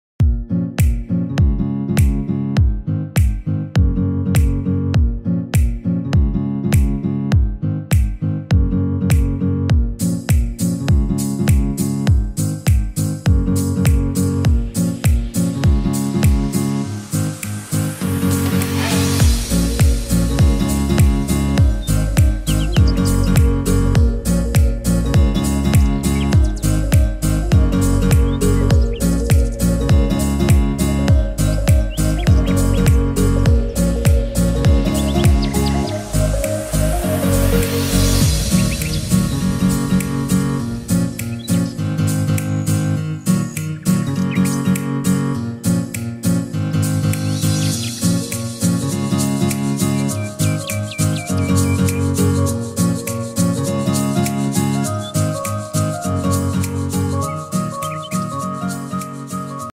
Tutorial espejo DIY con marco de puerta de madera antigua Aprende a hacerlo
Sencillo tutorial para realizar un espejo con el marco de una puerta antigua! Recicla la madera y decora tu hogar con mucho estilo.
Las herramientas conseguilas en molber